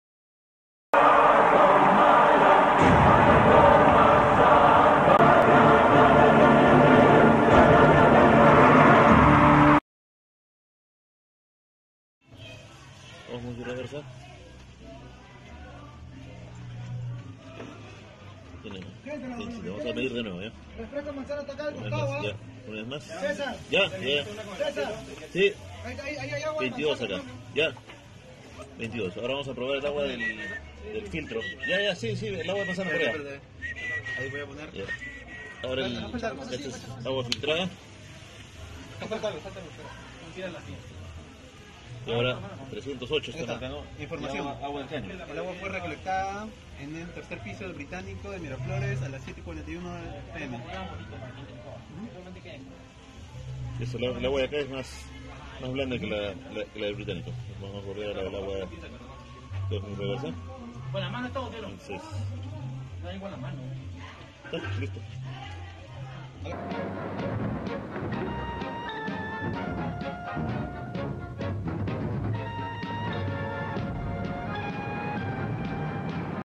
Medición de tipos de agua para el uso en plantas carnívoras
En este video se toma en cuenta las mediciones de algunos tipos de agua como la de osmosis inversa ,filtrada y la de caño para determinar cual de ellas sería la mejor para usarla en nuestras plantas carnívoras considerando los valores del ppm